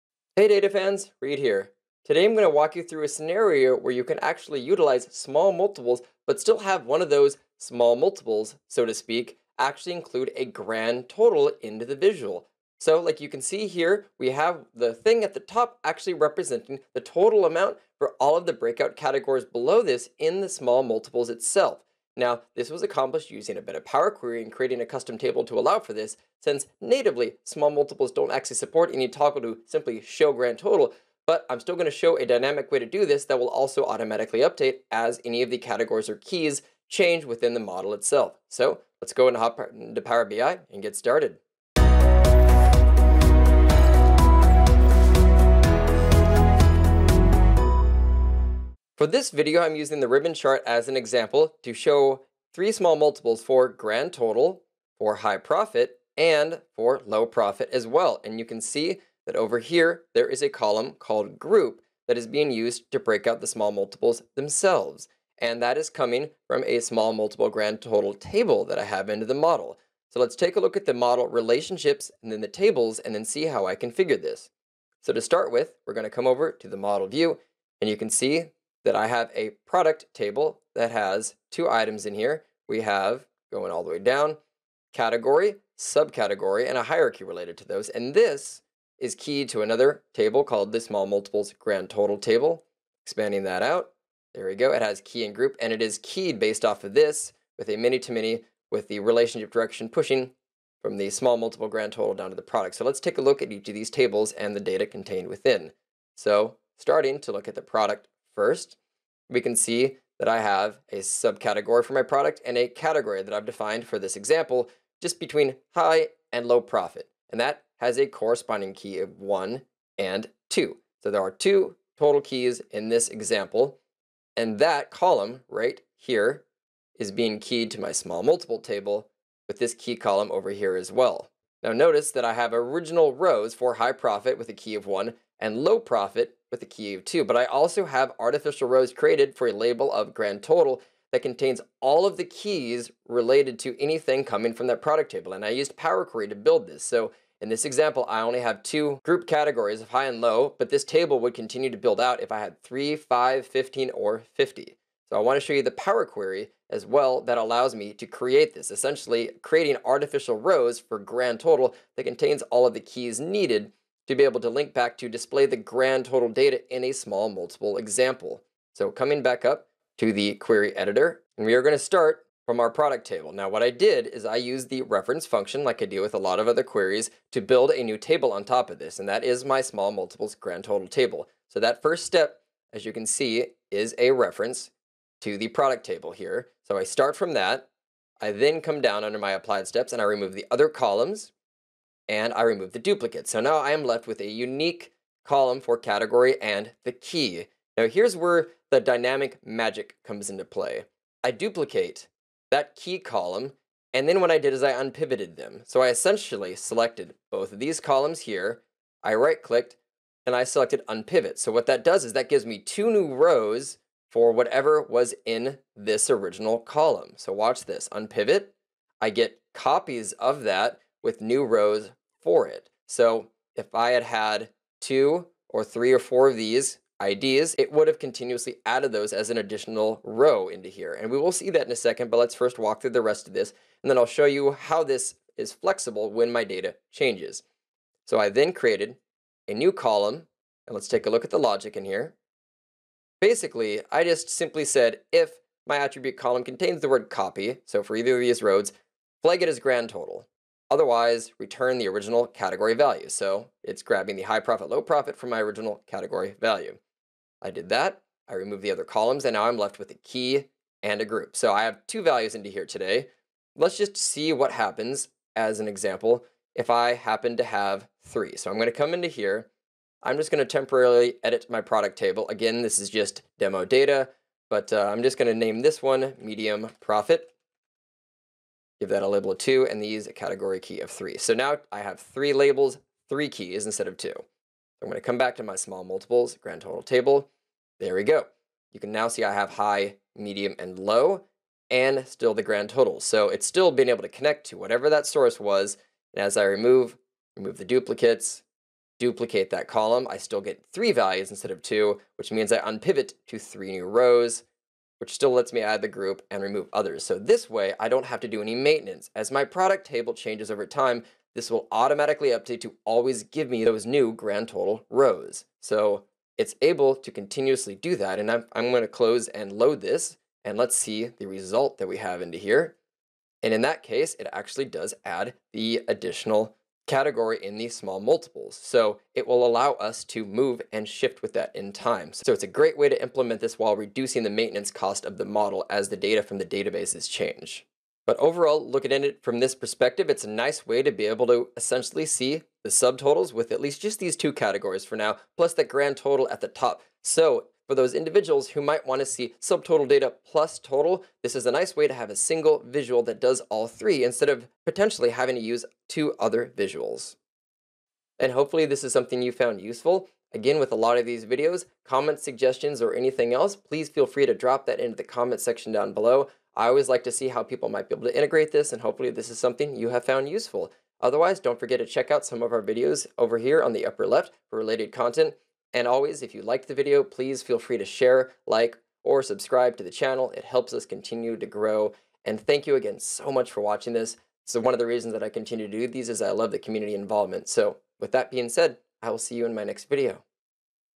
Adding Grand Totals to Small Multiples in Power BI
Learn how to configure your model to enable a Grand Total (multiple) when adding small multiples to native visuals in Power BI. There's a mix of modeling and Power Query (M)agic involved. So tune in to learn more!

VIDEO CHAPTERS 🎥
0:00 - Start of Video
0:49 - Model Walkthrough
2:49 - Power Query Walkthrough